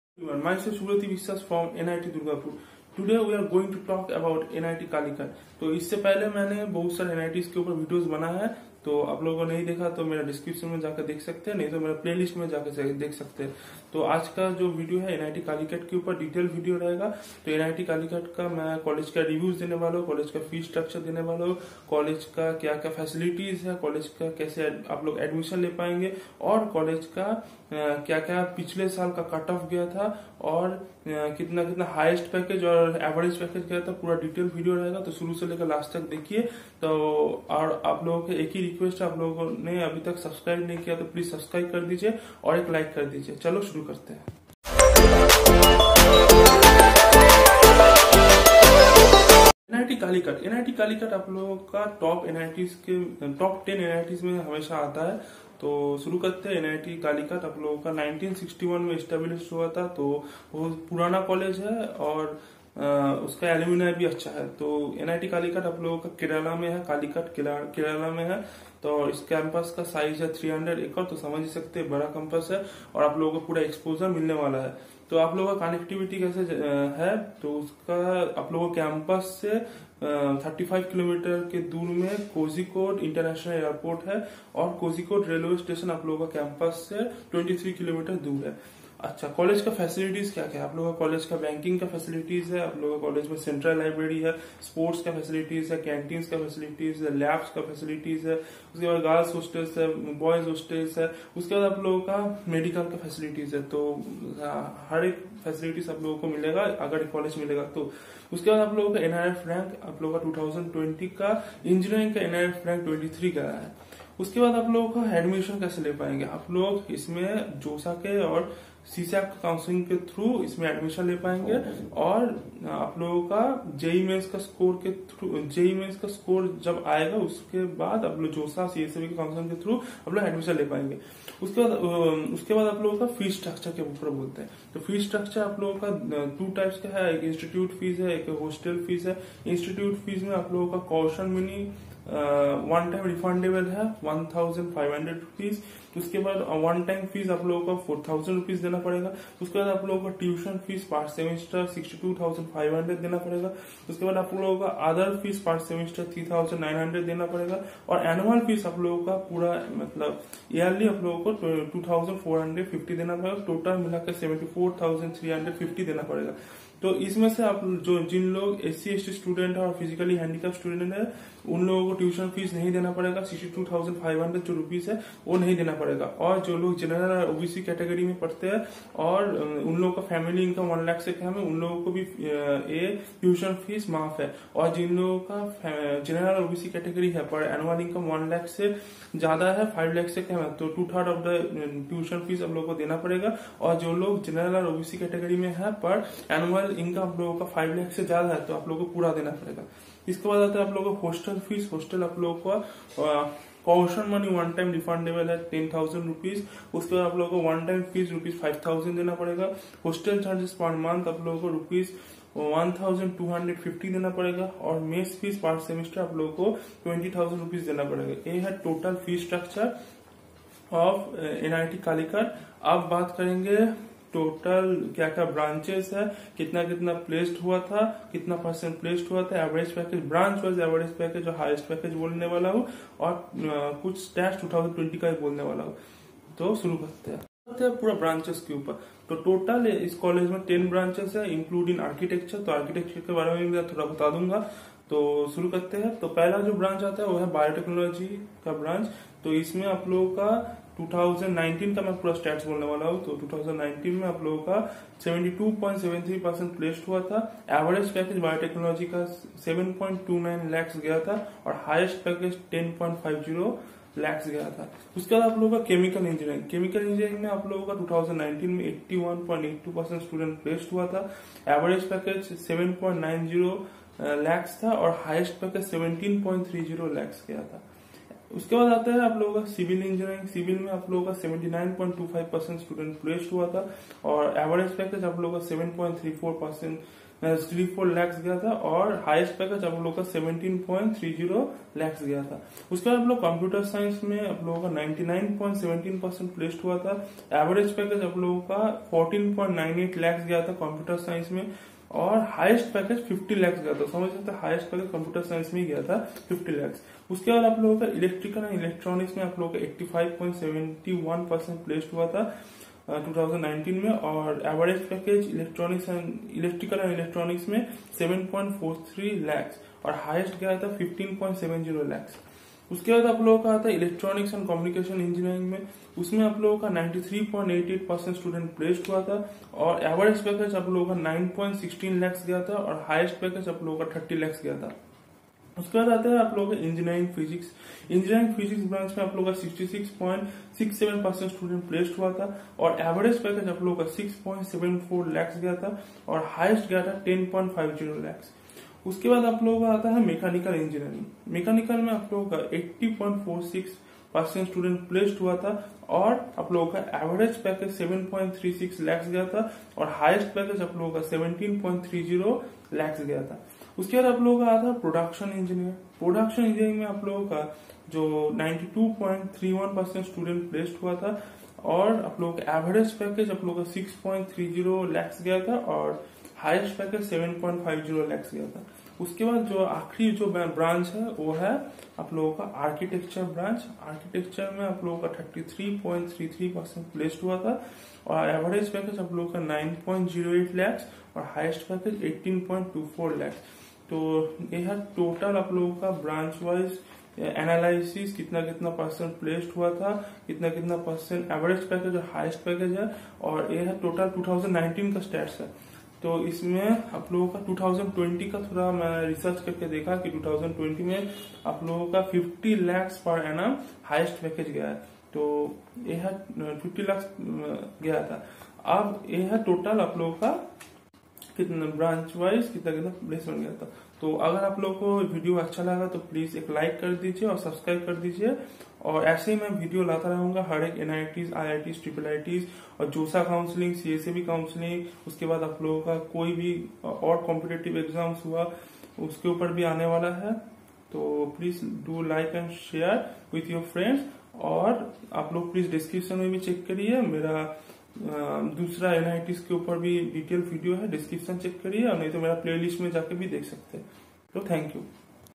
Detailed analysis of NIT Calicut
Today we here discuss detailed analysis of nit Calicut, Top NITs in India, we talk here about college branch wise placement , average and highest packages offered by the company in the year 2019 and 2020, we also discussed about fees structure of NIT Calicut , we make a effort to provide a detailed analysis of NIT Calicut report , It always stands in top 10 NITs of India and it is a prestigious institute and for taking admission into it we have to participate through jossa and csab counselling , full detailed manner it has been described .